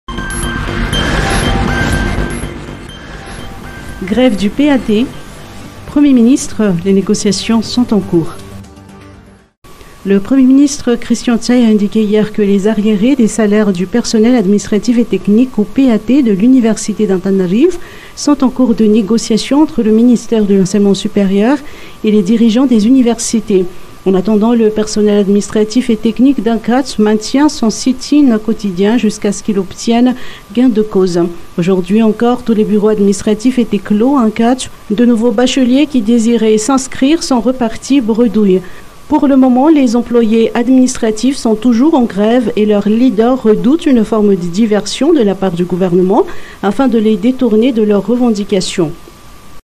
4 - 23/10/2021
Le Premier ministre christian ntsay a indiqué hier que les arriérés des salaires du personnel administratif et technique ou PAT de l’université d’Antananarivo sont en cours de négociations entre le ministère de l’enseignement supérieur et les dirigeants des universités.